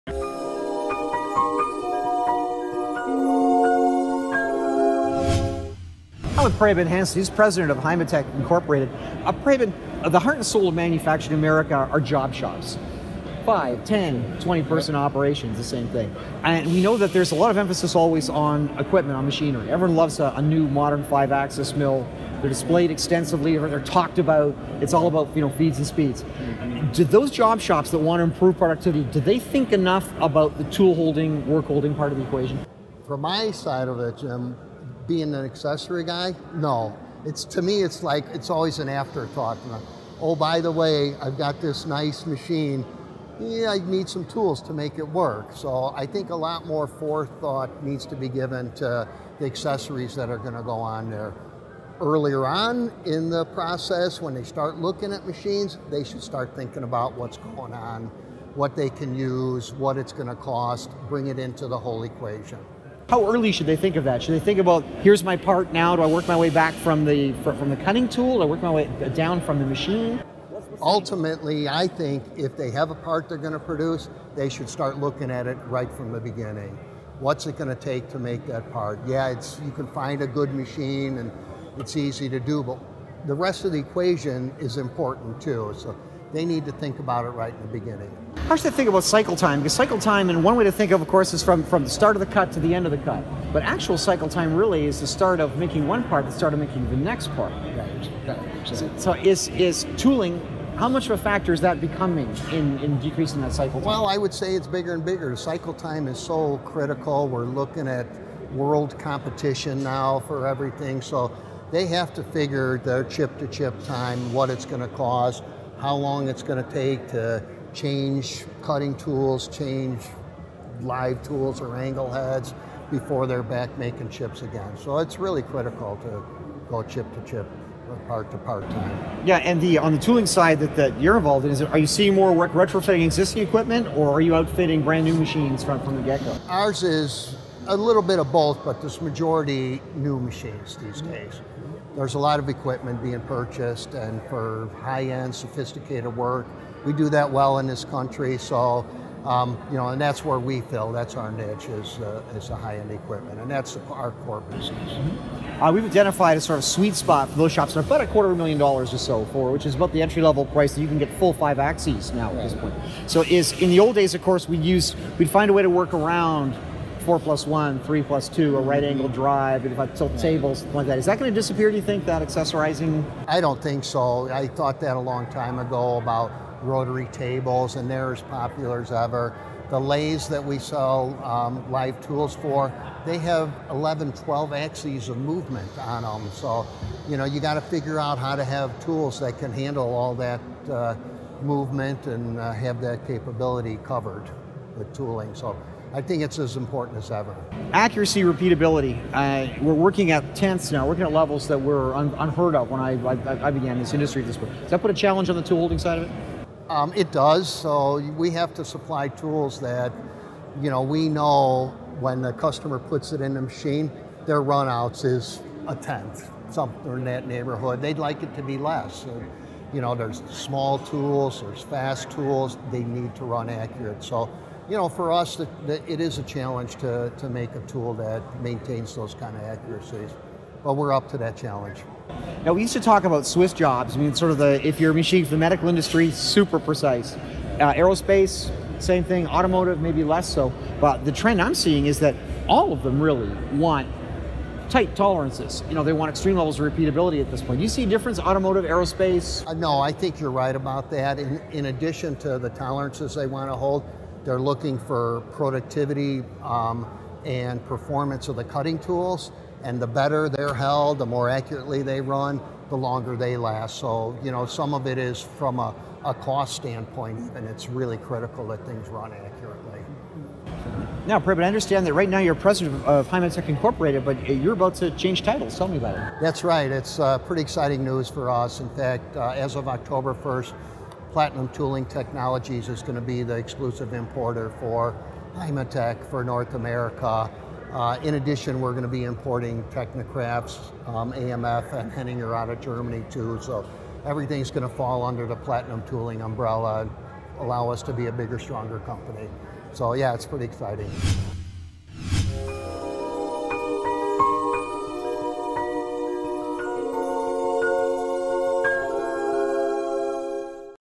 Why Small Job Shops are Buying High-Tech Five Axis CNC Machines
Five-axis machining, whether it's a 3+2 machine or simultaneous, is an expensive technology, but it's not just for big, deep-pocketed companies.

Please note: Heimatec. Inc. is now known as Platinum Tooling, Inc.